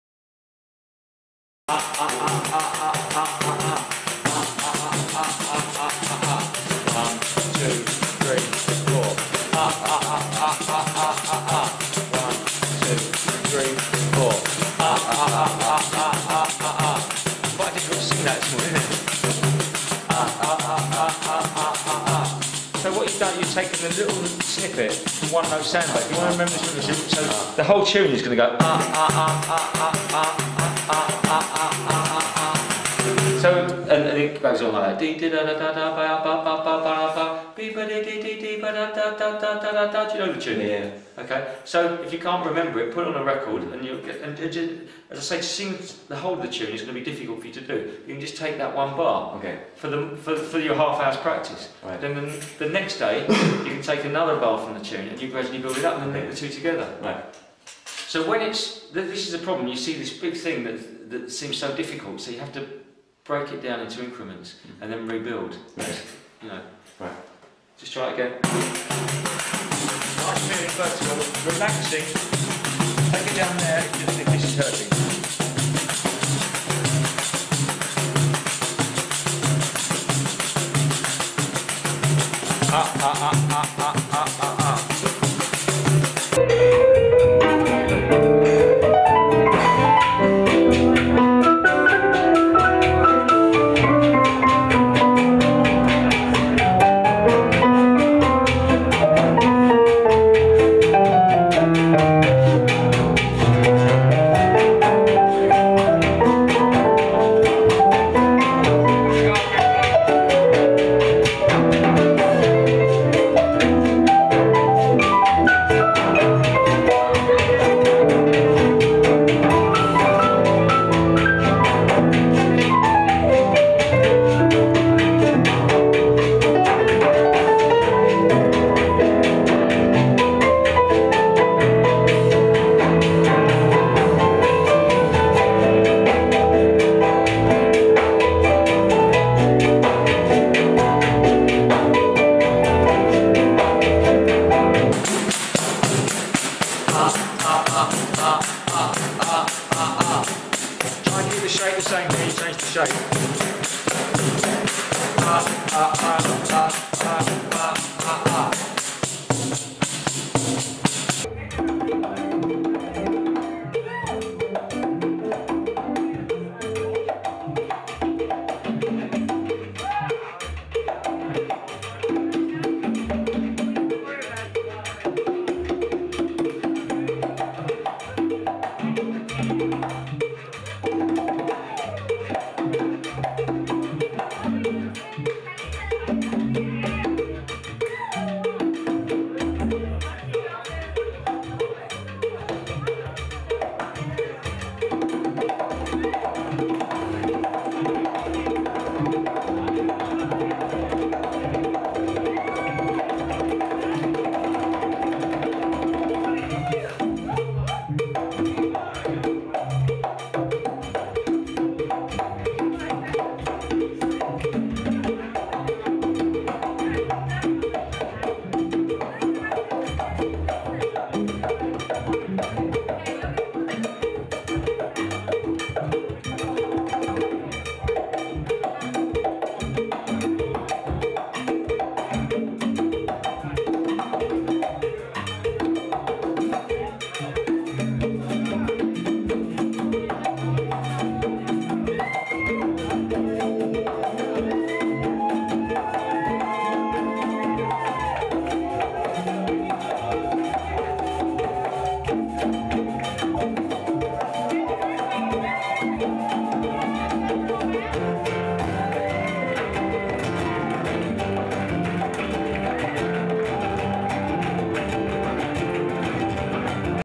Paul Clarvis at SJP 2010
Sligo Jazz Project Summer School Percussion Tutor Paul Clarvis in action at the 2010 event. One of the top UK session drummers, Paul is an also an experienced latin, orchestral & afro-cuban percussionist and was Leonard Bernstein's favourite percussionist when he came to the UK! He's played with everyone from Mick Jagger to Nina Simone and is a great guy to boot! Some highlights of Sligo Jazz Project 2010 included his workshops and wonderful appearances at our concerts and jam sessions.